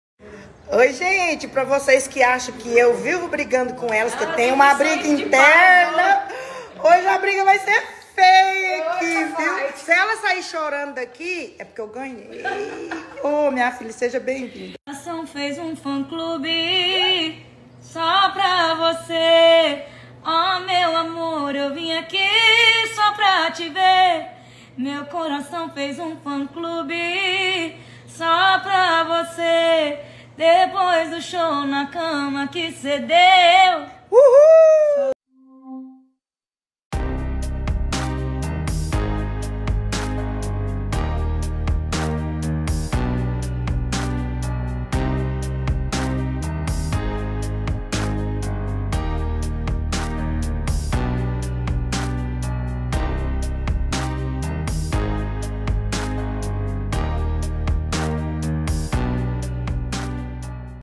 Maiara canta para o filho de Marília Mendonça e emociona fãs
Maiara, da dupla Maiara e Maraisa, fez uma visita tocante a Dona Ruth, mãe de Marília Mendonça, nesta sexta-feira (26). A cantora, que compartilhou uma amizade próxima com Marília até sua trágica partida em 2021, encantou o filho da colega, Léo, ao cantar para ele.